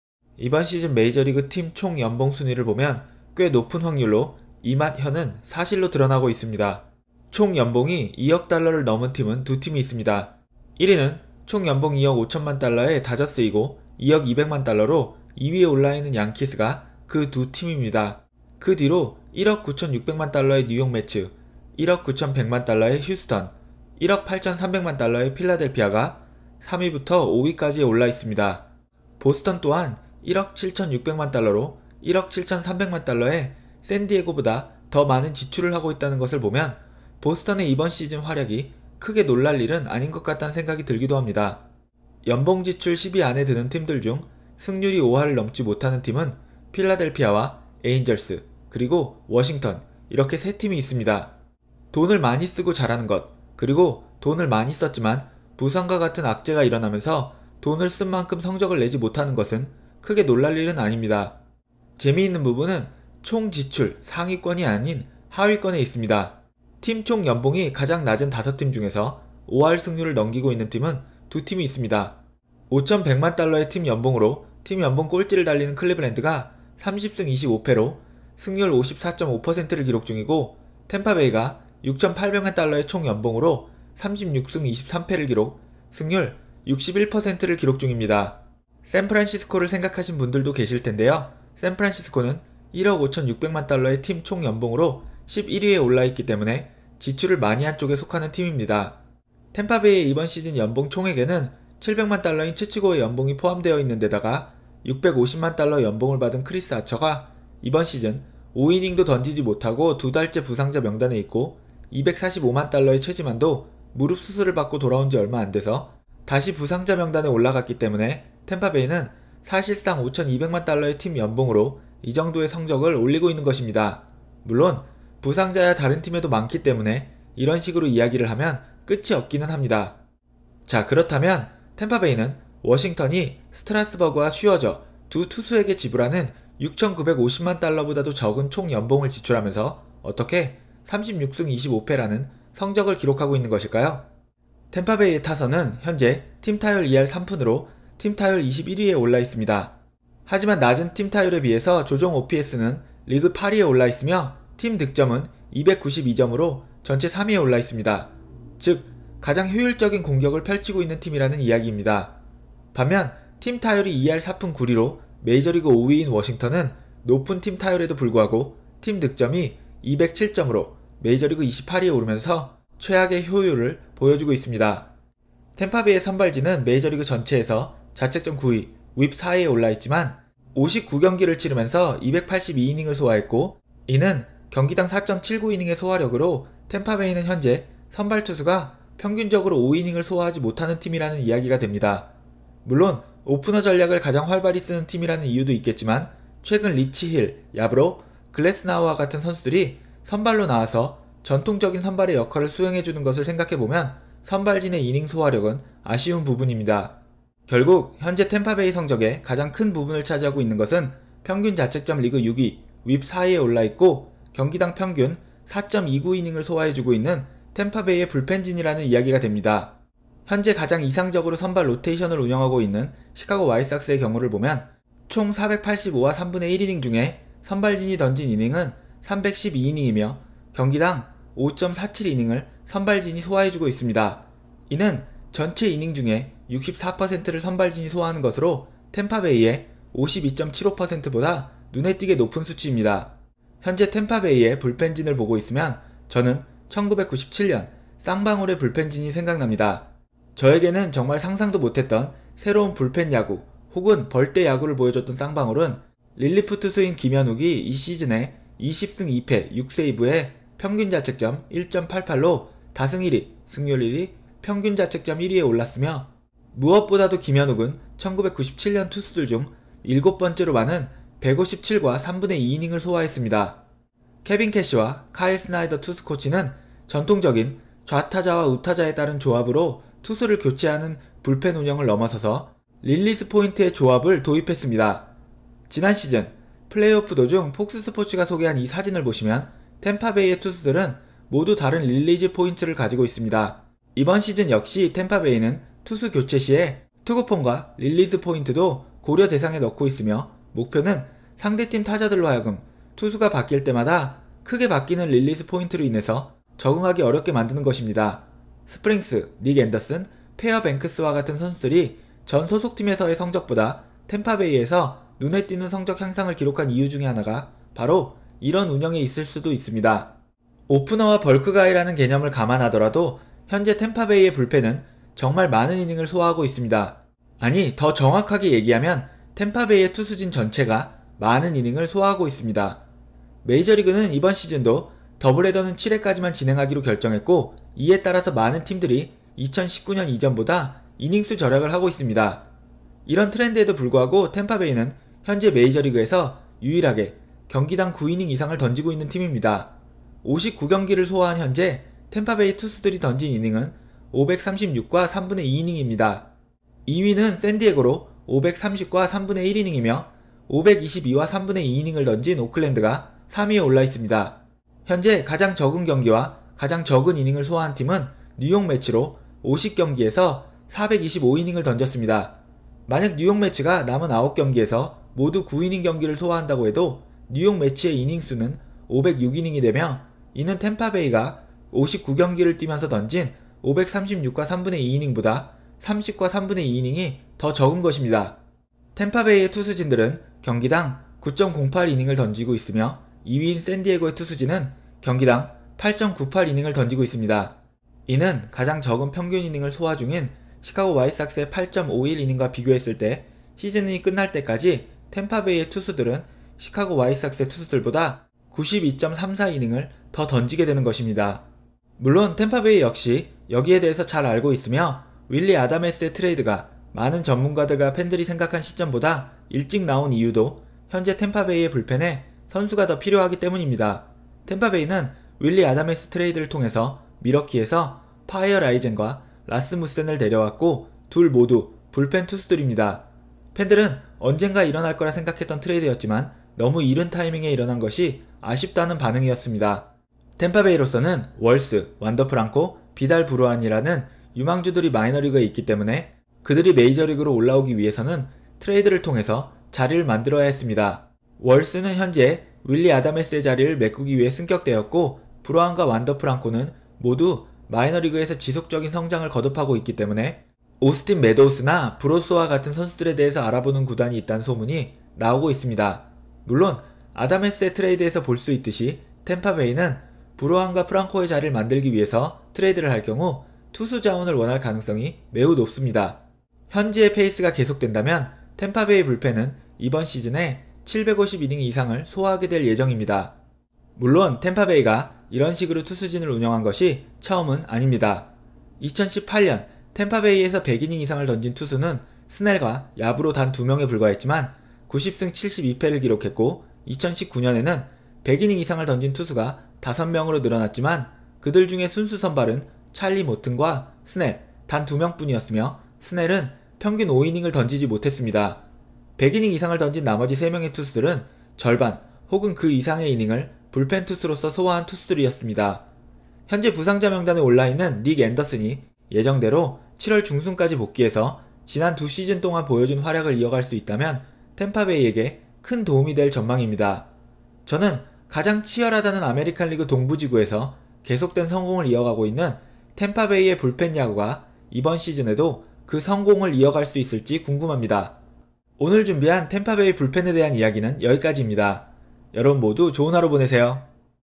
'아메리칸리그 1위' 탬파베이의 환상적인 투수 운영 | 이승용 MLB
연봉 총액 하위권 탬파베이가 잘하는 이유